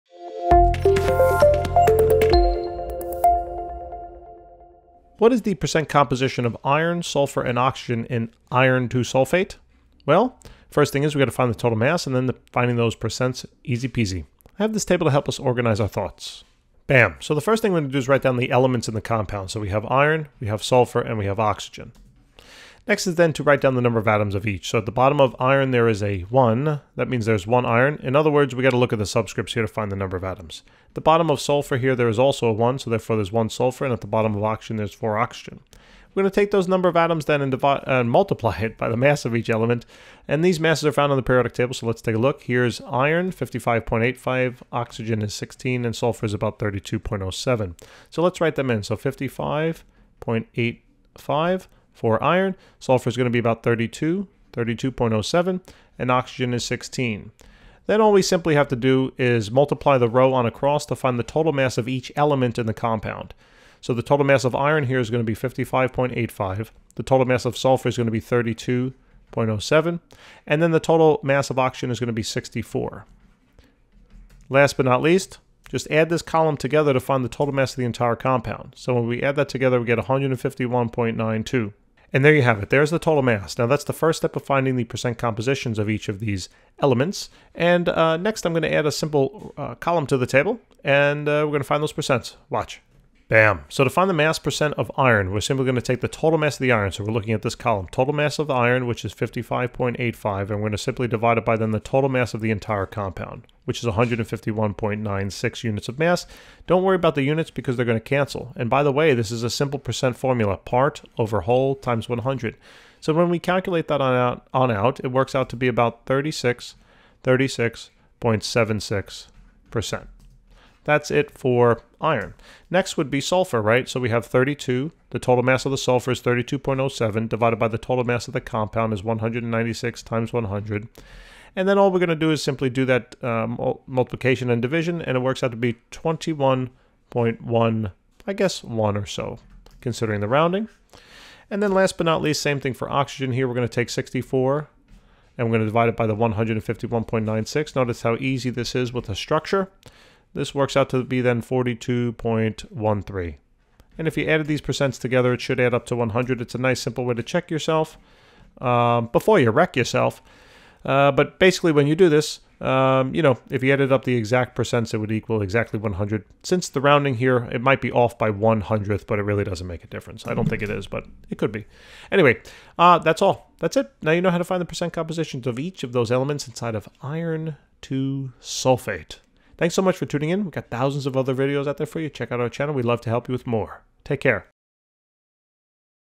How to find the percent composition of FeSO4 (Iron (II) Sulfate)
Calculate the percent composition of the following:
FeSO4 (Iron (II) Sulfate)